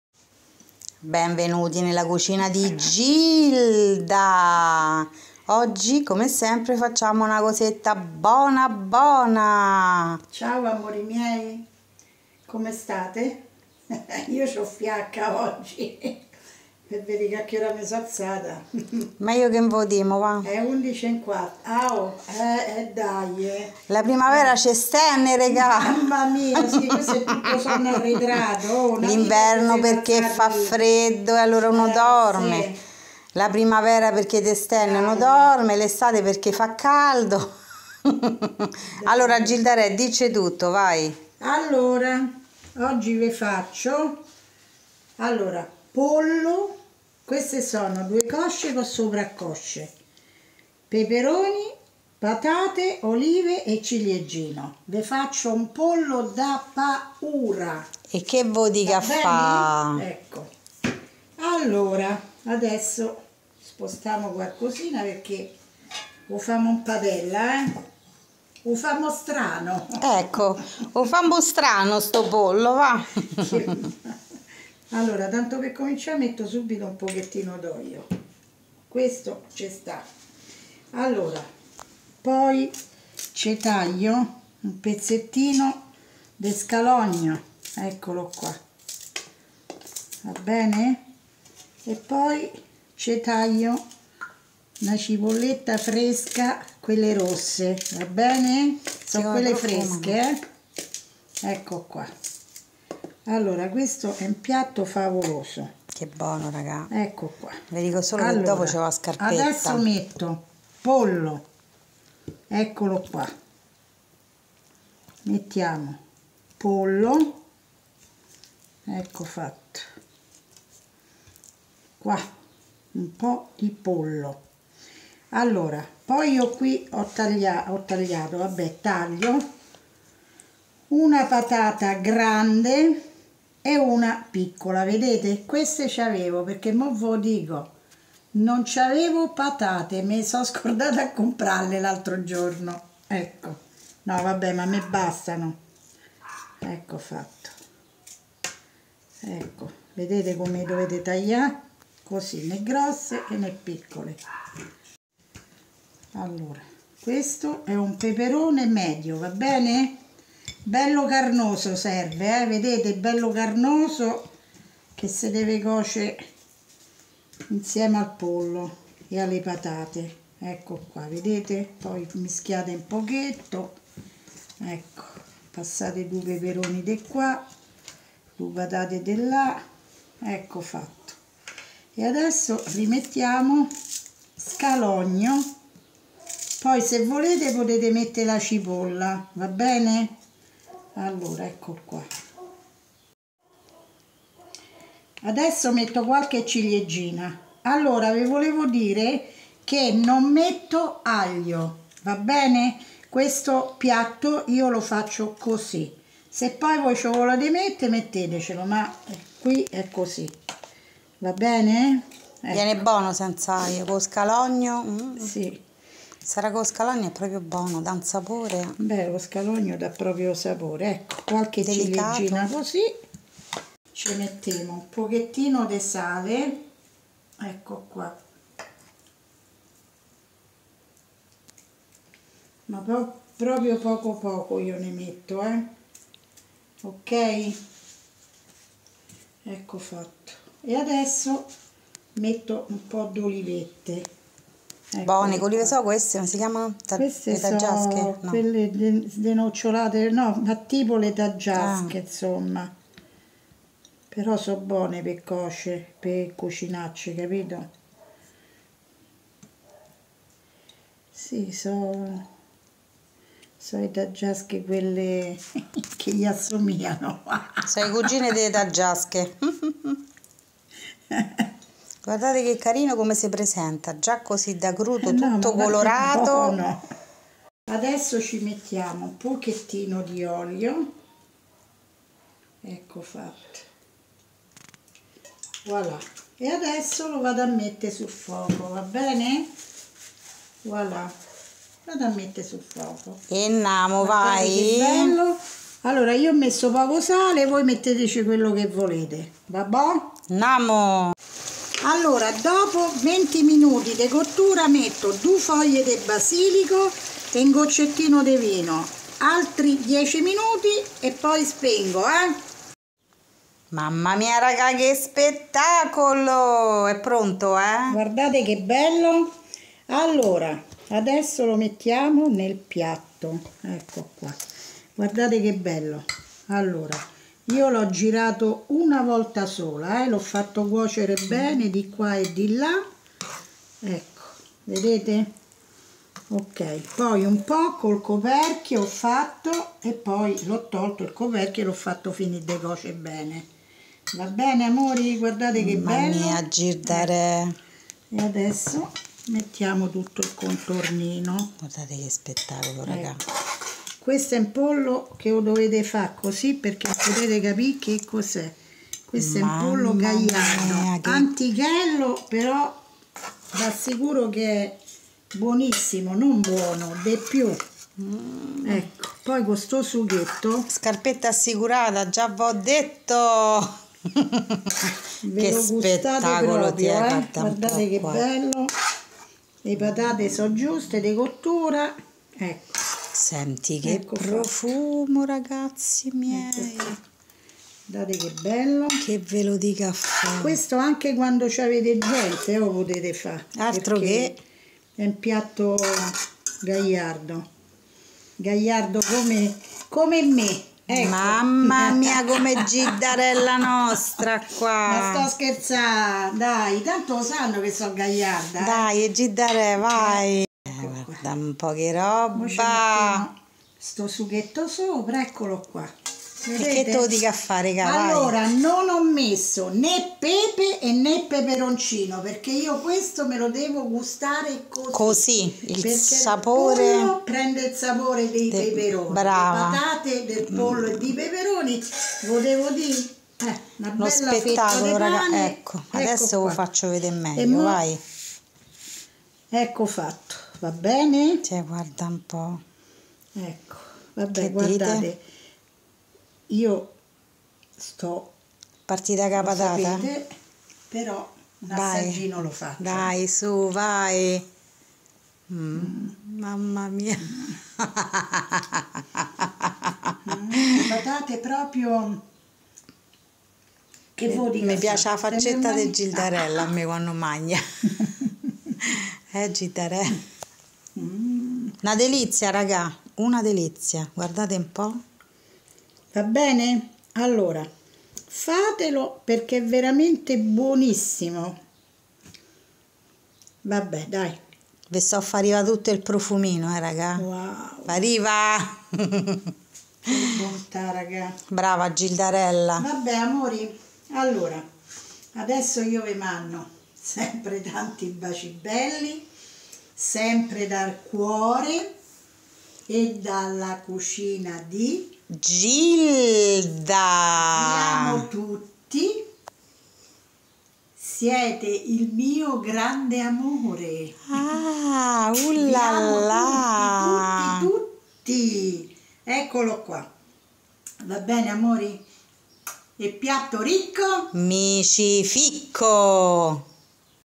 Pollo con peperoni e patate: ricetta tradizionale per un succulento secondo piatto 🫶
📌 Ingredienti:
2 cosce pollo
2 sovra cosce pollo
2 patate
1 peperone medio
10 pomodorini ciliegino
100g olive leccino denocciolate
2 scalogni piccoli
1 cipolletta fresca rossa
qb basilico fresco
1 goccio di vino
qb olio extravergine
qb sale